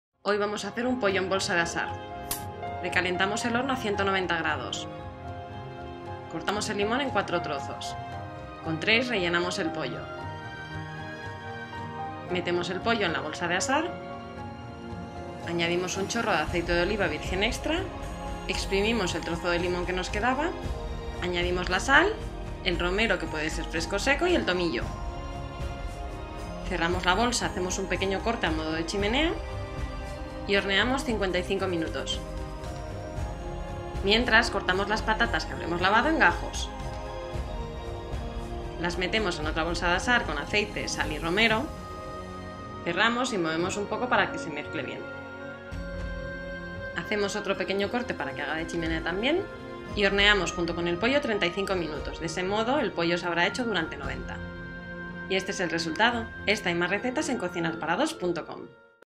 Pollo de corral en bolsa de asar
Receta para cocinar el pollo cómodamente y de forma limpia en una bolsa de asar.

INGREDIENTES
• 1 pollo de corral entero
• aceite de oliva virgen extra (AOVE)
• sal
• 1 limón
• tomillo, romero...
• 4 patatas pequeñas nuevas con la piel fina
Además necesitaremos
• 2 bolsas de asar

ELABORACIÓN
Precalentar el horno a 190º.
Trocear el limón en 4 partes y rellenar el pollo con 3 de los trozos. Meter en la bolsa de asar. Añadir un chorro generoso de aceite, el jugo del trozo de limón que hemos reservado, sal y tomillo y romero (las hierbas pueden ser frescas o secas).
Cerrar la bolsa con el precinto. Hacer un agujero pequeño en la bolsa para que haga de chimenea.
Hornear a 190º durante 55 minutos (en total serán 90, pero será en dos tandas)
Mientras, cortar las patatas en gajos una vez lavadas.  Meter en la otra bolsa con un chorro de aceite, sal y romero.  Cerrar con el precinto, hacer otro agujero para que haga de chimenea. Cuando hayan pasado los 55 minutos, meter la bolsa de las patatas en el horno y cocinar durante 35 minutos más.